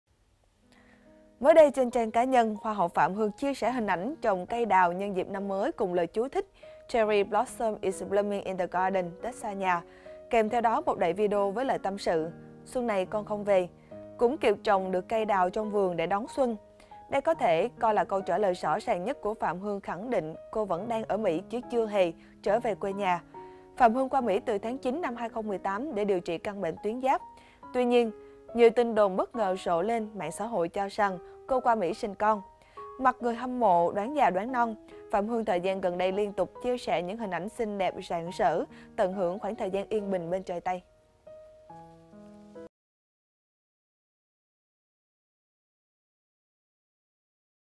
Giữa nghi án sang Mỹ dưỡng thai, Phạm Hương nói gì?
VTC9 | Đứng trước tin đồn sang Mỹ dưỡng thai, sinh con cho đại gia, Hoa hậu Hoàn Vũ Việt Nam 2015 Phạm Hương tỏ ra bình tĩnh thậm chí có phần lạc quan.